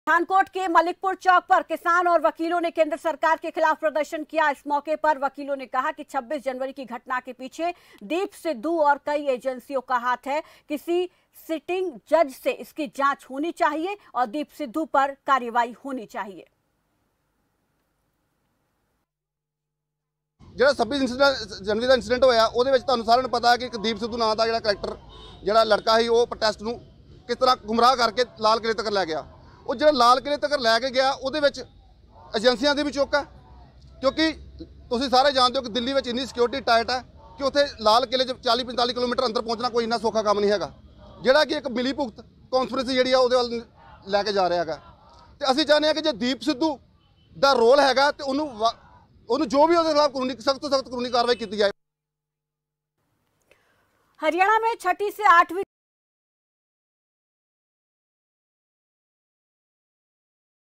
PUNJAB- किसानों के समर्थन में वकीलों का 'हल्लाबोल | Khabar Fast News
PUNJAB- किसानों के समर्थन में वकीलों का 'हल्लाबोल'
'26 जनवरी की घटना के पीछे कई एजेंसियों का हाथ'